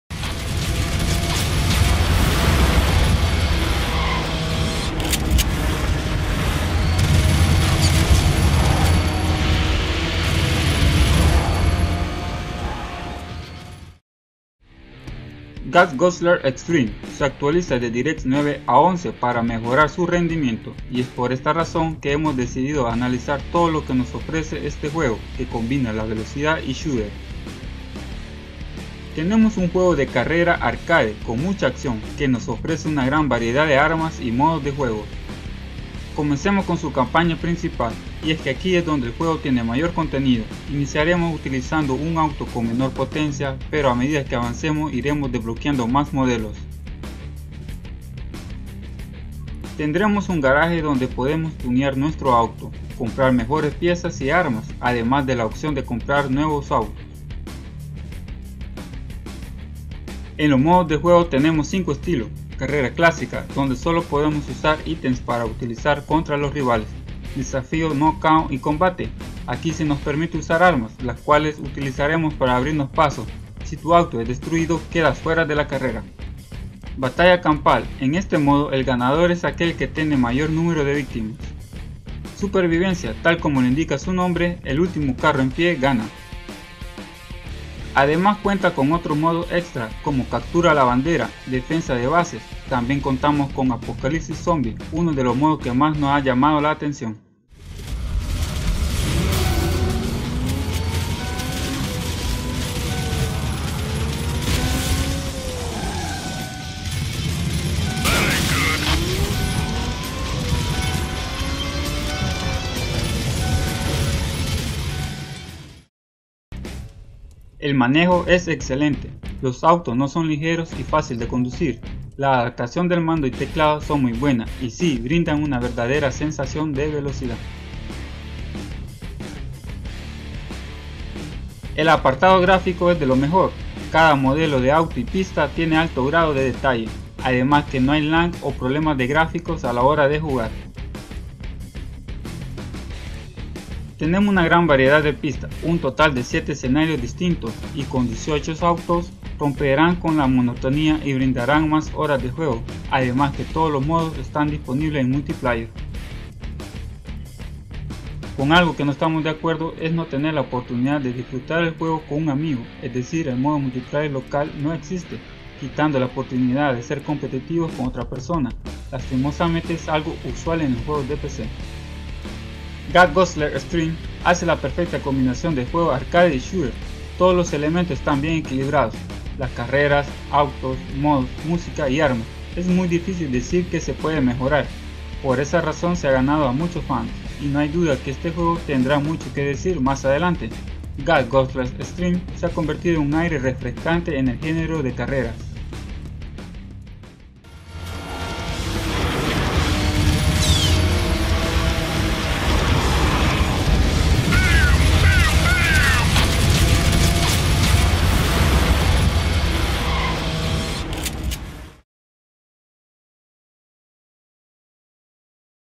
Gas Guzzlers Extreme Review
Gas Guzzlers Extreme se actualiza a directx 11 sacando lo mejor del juego, el cual es una combinacion de carreras arcade y shooter con multiples modos de juegos.